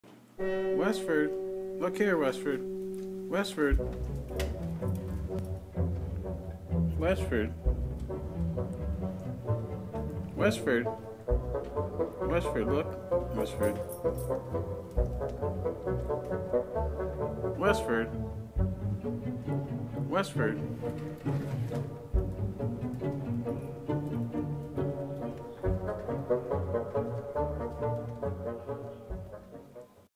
MK ULTRA BRAINWASH YOUR DOG.....just keep saying the same things over & over/works for people:-)@
See how he has his own mind.he cannot be swayed EVEN by the MOUNTAIN K.innetic. I.nfluence.Narrator.G.rammer...must study further.i must learn these skills